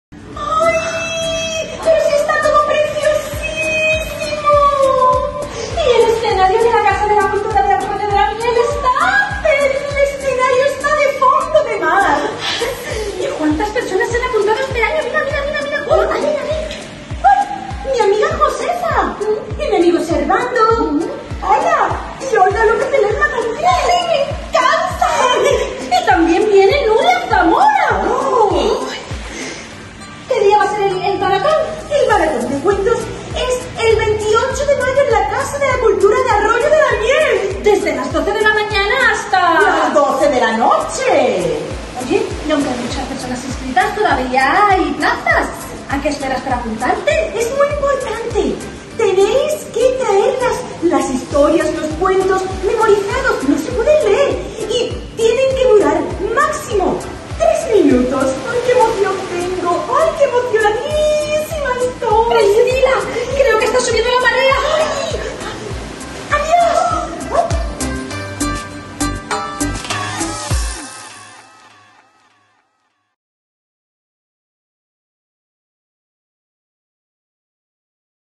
Maratón de Cuentos de Benalmádena 2022
Spot publicitario del Maratón de Cuentos de Benalmádena, que tiene lugar todos los años en la Casa de la Cultura.
Este año se celebra el sábado día 28 de mayo de 2022.